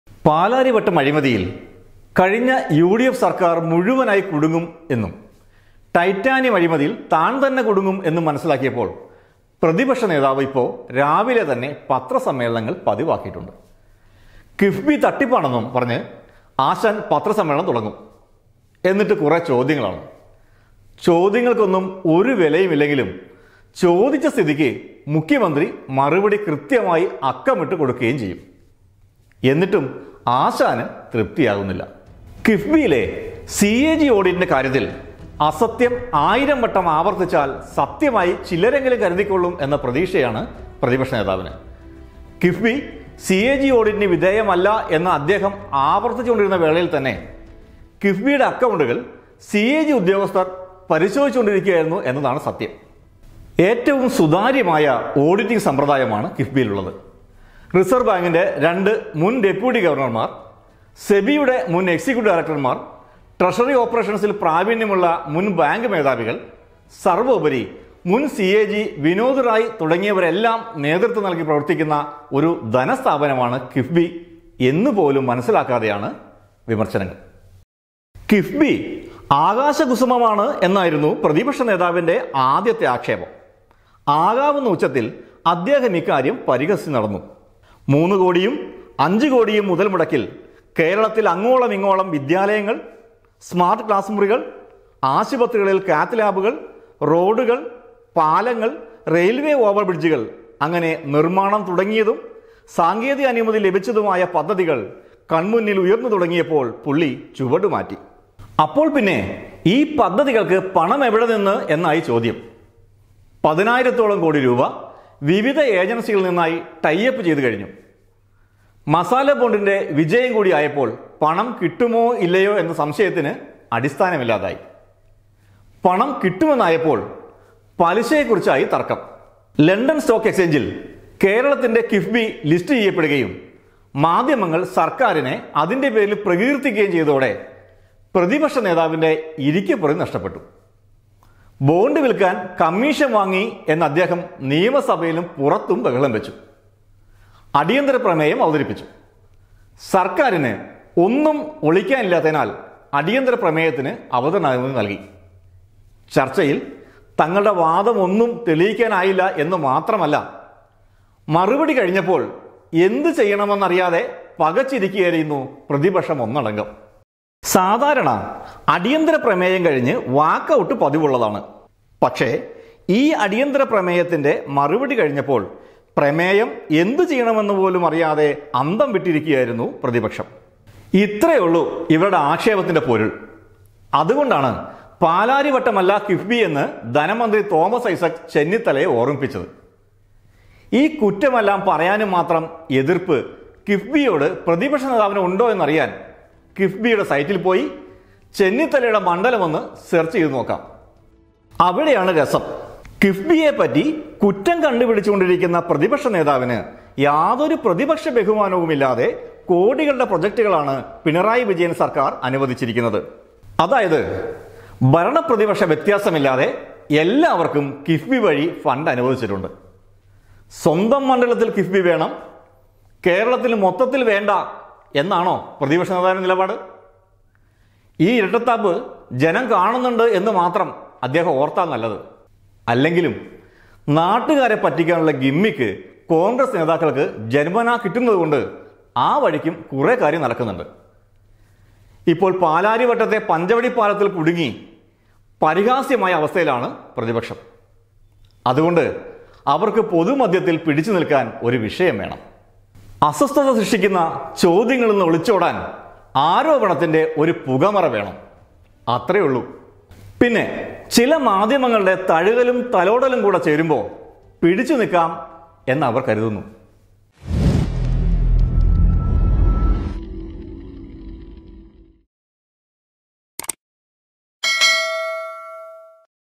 എന്റെ ചെന്നിത്തലേ പാലാരിവട്ടമല്ല കിഫ്ബി | KIIFB | Ramesh Chennithala
കിഫ്ബിയിലെ സിഎജി ഓഡിറ്റിന്റെ കാര്യത്തില്‍ അസത്യം ആയിരം വട്ടം ആവര്‍ത്തിച്ചാല്‍ സത്യമായി ചിലരെങ്കിലും കരുതിക്കൊള്ളും എന്ന പ്രതീക്ഷയാണ് പ്രതിപക്ഷ നേതാവിന്.